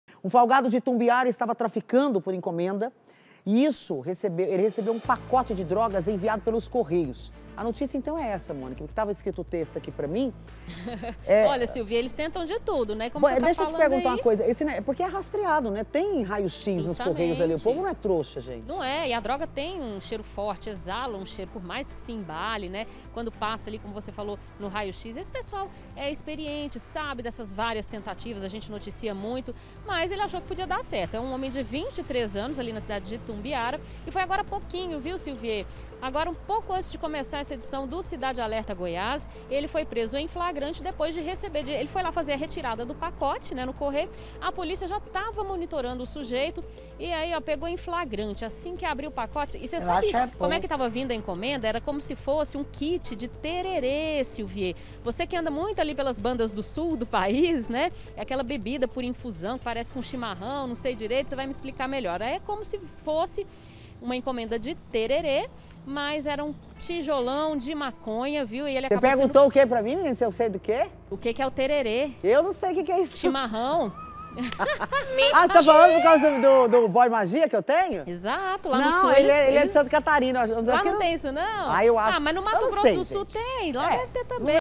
TRÁFICO POR ENCOMENDA PRESO RECEBEU PACOTE DE DROGA PELOS CORREIOS
UM FOLGADO DE ITUMBIARA ESTAVA TRAFICANDO POR ENCOMENDA ISSO MESMO RECEBEU UM PACOTE DE DROGAS ENVIADO PELOS CORREIOS.